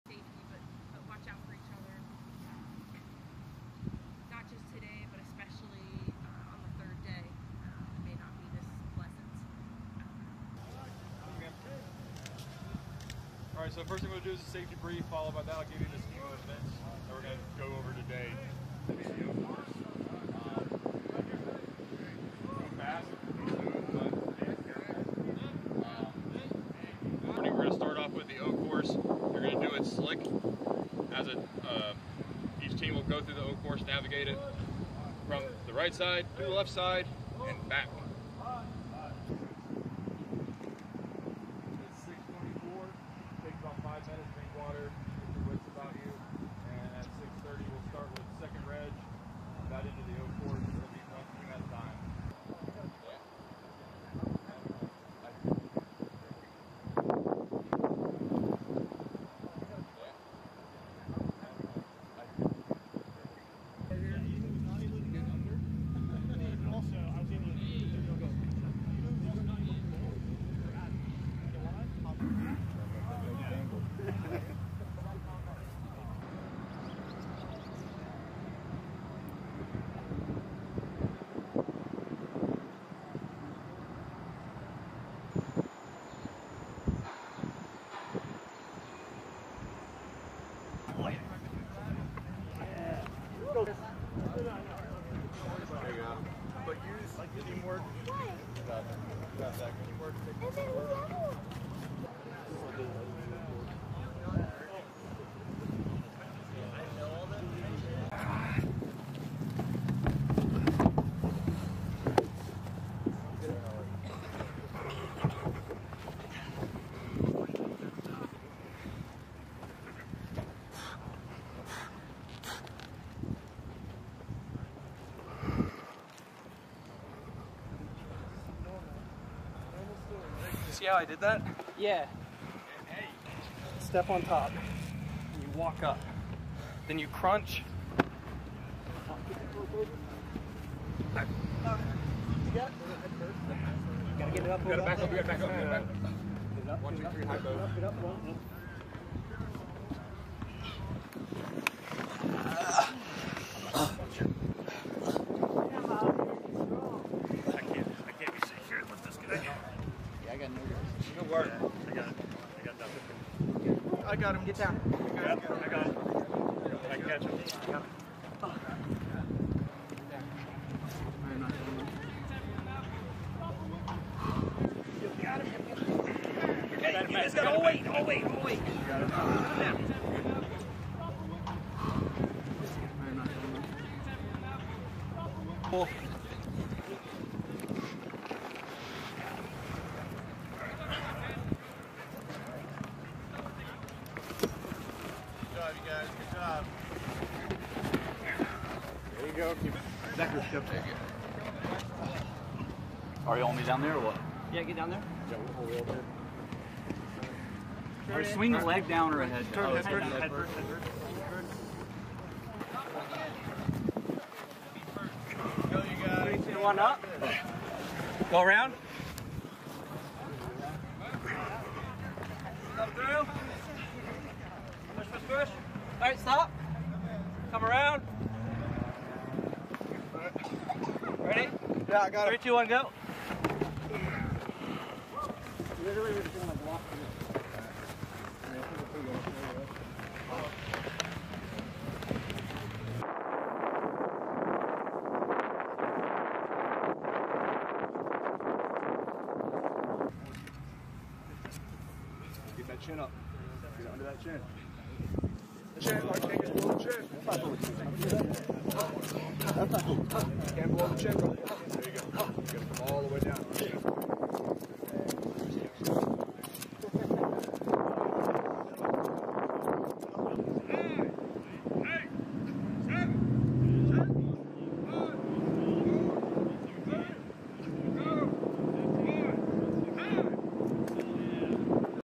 U.S. Navy Sailors from 2d Marine Division compete in the Doc Kent Competition | B-roll Day 1
U.S. Navy Sailors from 2d Marine Division compete in the Doc Kent Competition on Camp Lejeune, North Carolina, Aug. 2, 2022.

Doc Kent Competition B-roll Day 1
CAMP LEJEUNE, NC, UNITED STATES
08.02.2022

The competition showcases the capabilities of executing tactical combat casualty care in austere environments by utilizing various evacuation platforms to move from point of injury to higher echelons of care. (U.S. Marine Corps video by Lance Cpl. Ethan Robert Jones)

Film Credits: Video by Lance Cpl. Ethan R. Jones
2nd Marine Division